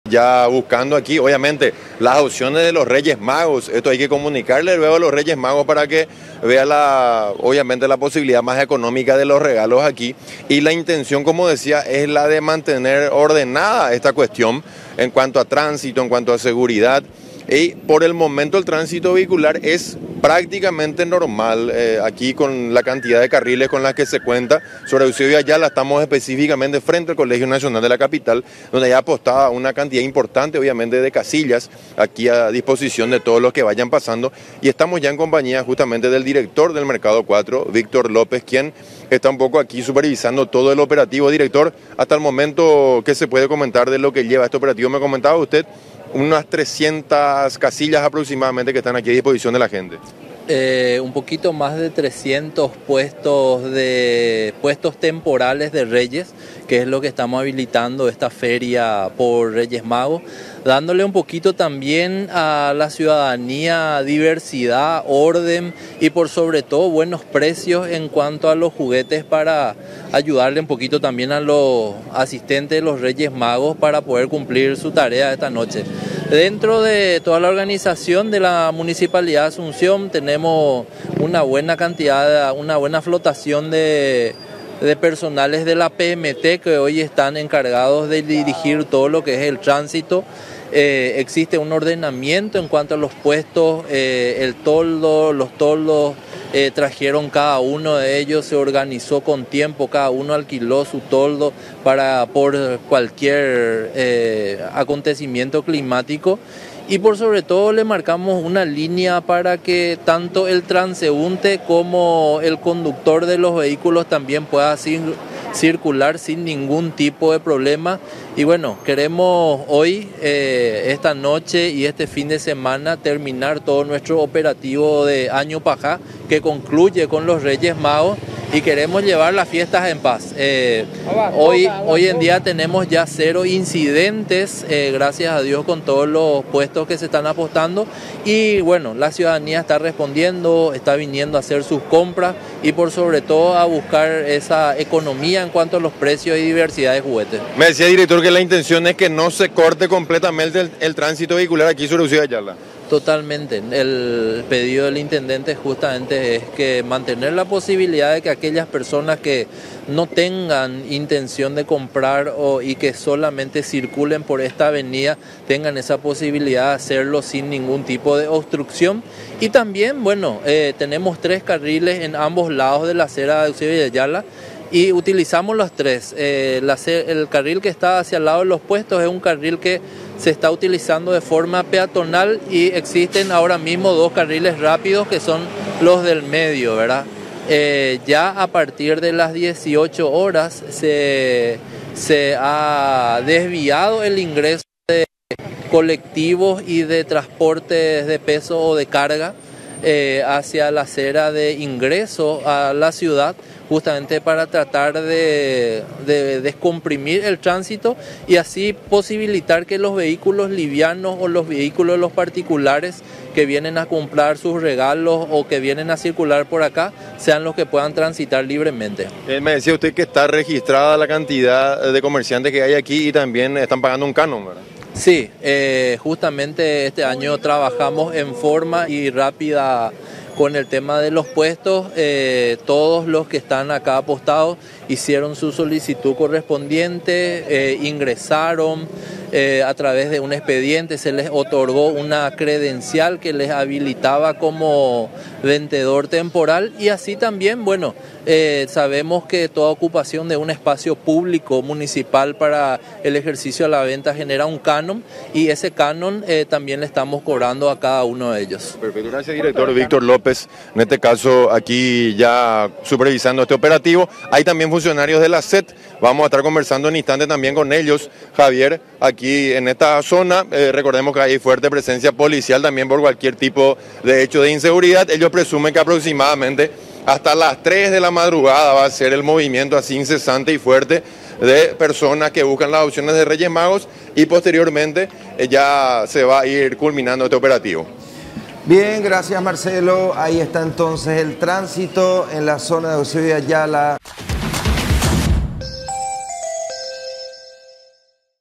Operativo Reyes Magos: caótico tránsito sobre Eusebio Ayala
Ante la feria instalada en la avenida Eusebio Ayala, a la entrada del Mercado 4, agentes policiales y municipales refuerzan la seguridad hoy por la llegada del 6 de enero. Uno de los carriles es de uso peatonal.